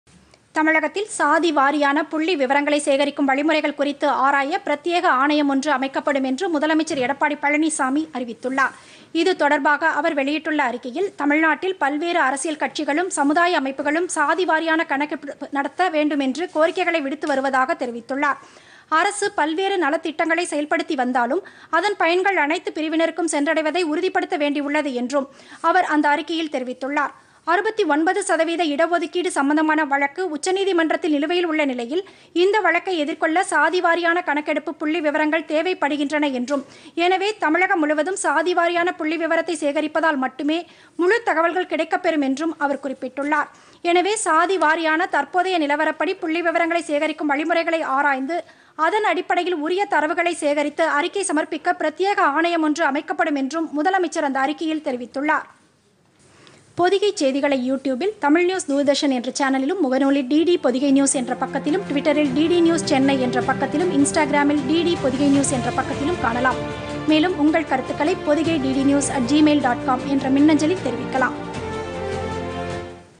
சாதிவாரி கணக்கெடுப்பு - ஆணையம் அமைப்பு
தமிழகத்தில் சாதிவாரி கணக்கெடுப்பு நடத்தி  புள்ளி விவரங்கள் சேகரிக்க  ஆணையம் அமைக்கப்படும் - முதலமைச்சர் எடப்பாடி பழனிசாமி அறிவிப்பு